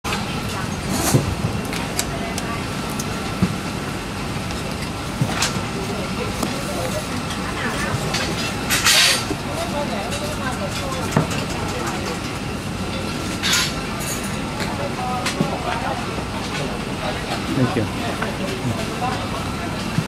Thai Corn sold by a street Vendor.....Bangkok, Thailand
Thai Corn sold by a street Vendor. Bangkok, Thailand. much of the corn is varigated and not the bright yellow corn that Westerners are used to. but this is a very tasty if chewy corn. yellow corn is also available but not as popular among the Thai.
The SONY NEX VG10 AF (auto-focus) had a hard time with this.